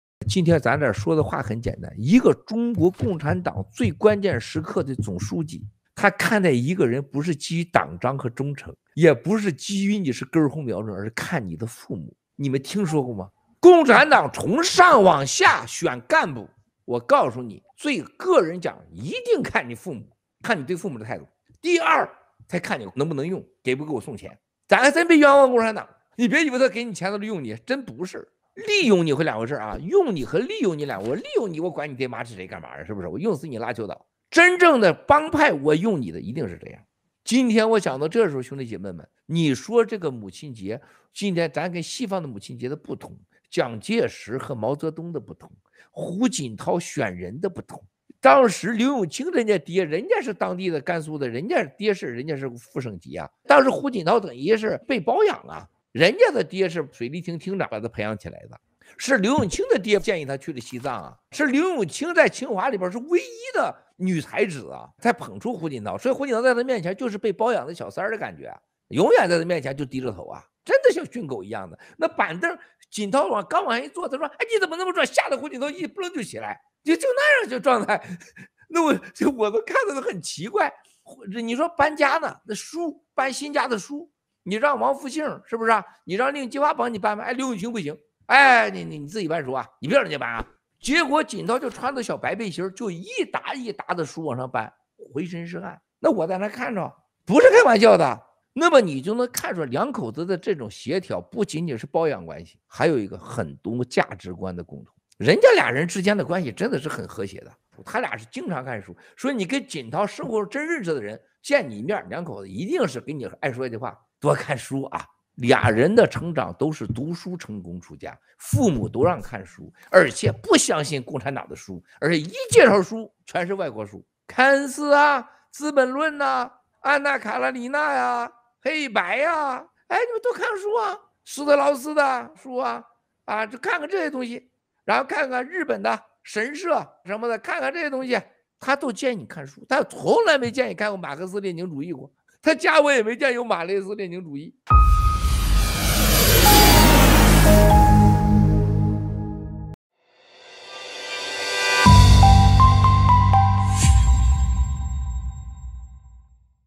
5/8/2022 文贵直播：中共选拔干部时，首先看他们对待父母的态度，其次才是能力和是否给钱贿赂；胡锦涛是如何上位的？他和夫人刘永清之间有什么样的关系？他们俩都爱读外国书，而家里却没有一本马克思。。。
5/8/2022 文贵直播：中共选拔干部时，首先看他们对待父母的态度，其次才是能力和是否给钱贿赂；胡锦涛是如何上位的？他和夫人刘永清之间有什么样的关系？他们俩都爱读外国书，而家里却没有一本马克思列宁主义的书

5/8/2022 Miles Guo: When the CCP selects cadres, it looks first at how they treat their parents, followed by the abilities and whether they bribe or not. How did Hu Jintao rise to power? What kind of relationship does he have with his wife Liu Yongqing? They both love to read foreign books, while there is not a single book of Marxism-Leninism in their residence.